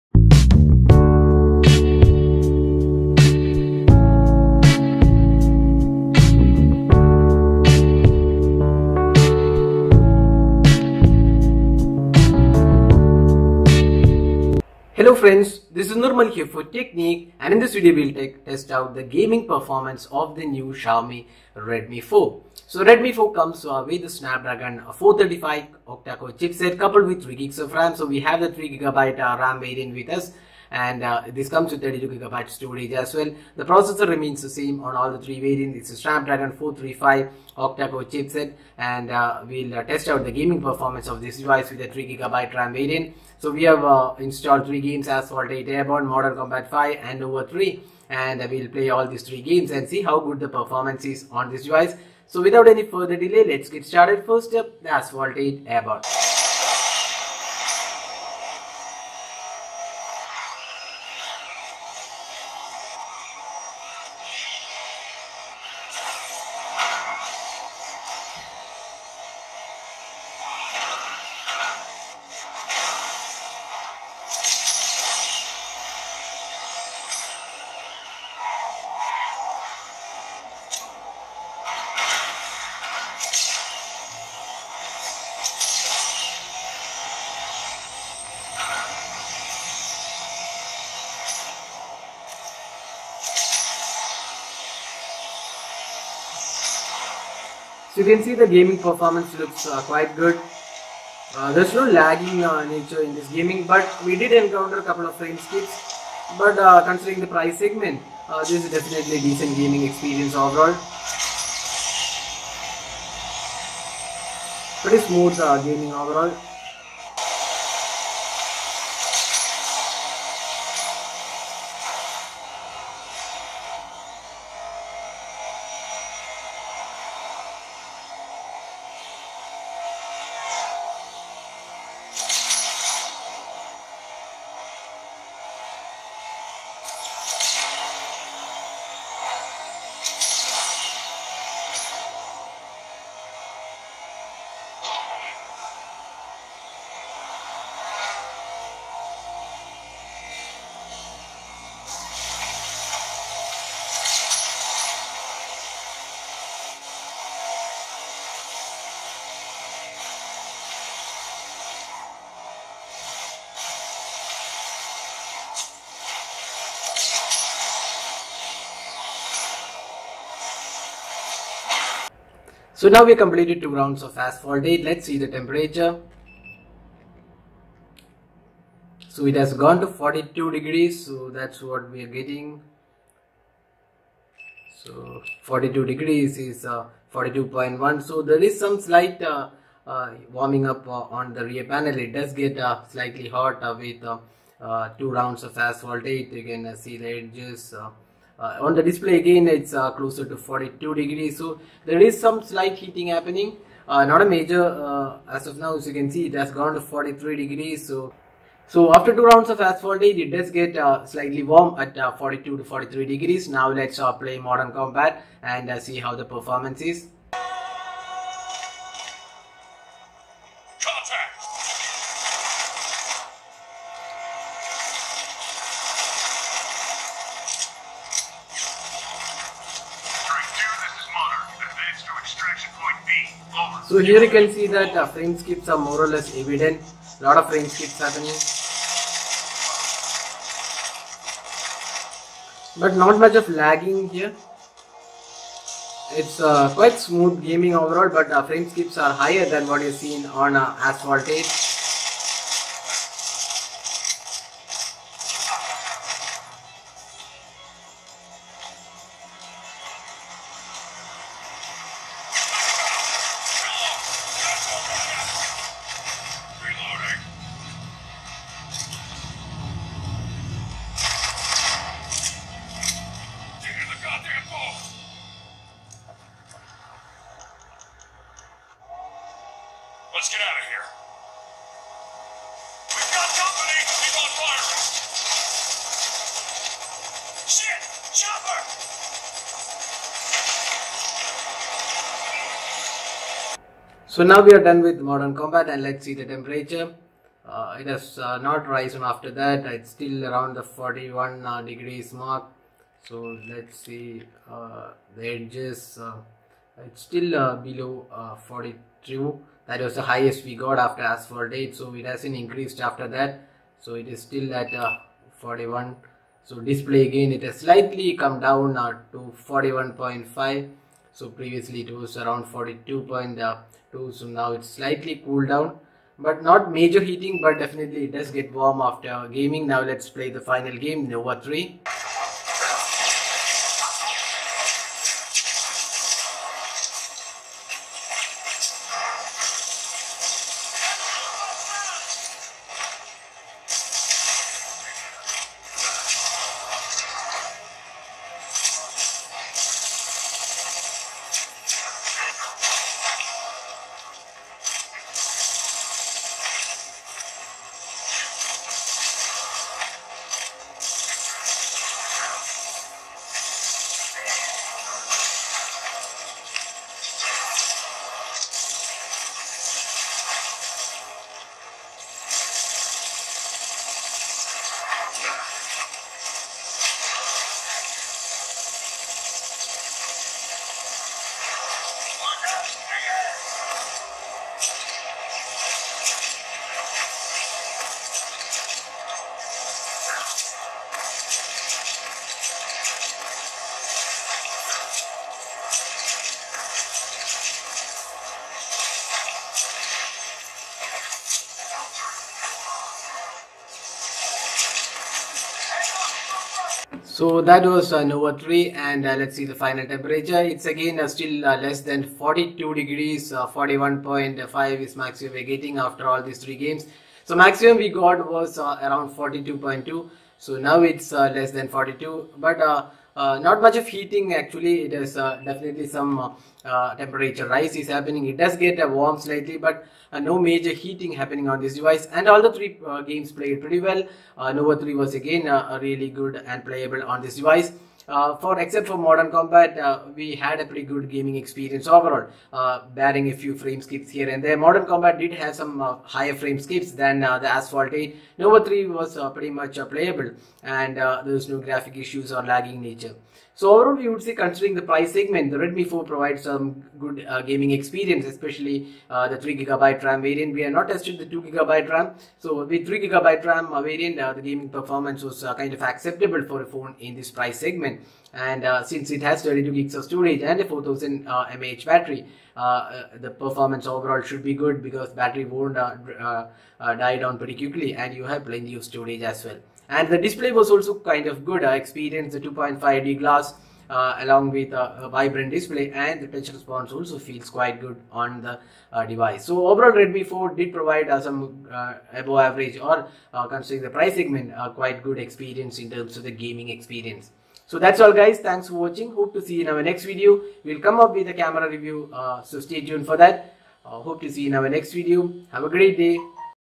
Xiaomi Redmi 4 (3GB RAM) Gaming Review with Heating Test
Xiaomi Redmi 4 comes with 5 inch HD display and is powered by Snapdragon 435 Octa core processor with 3GB RAM and 32GB storage. Here is the gaming performance review of the device along with heating test.